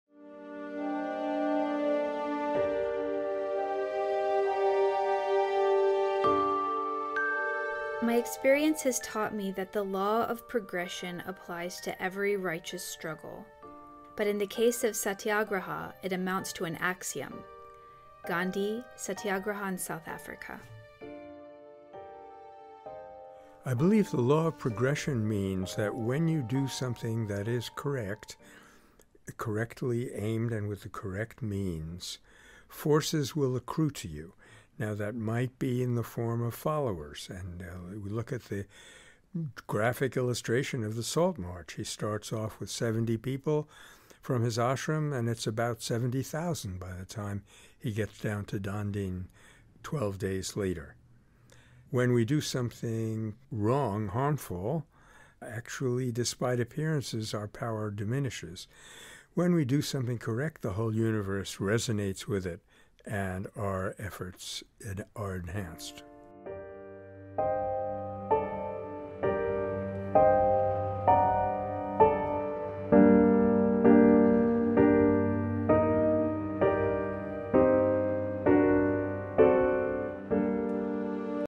Nonviolence Daily | Day 175
My experience has taught me that the law of progression applies to every righteous struggle. But in the case of Satyagraha it amounts to an axiom.
-Gandhi, Satyagraha in South Africa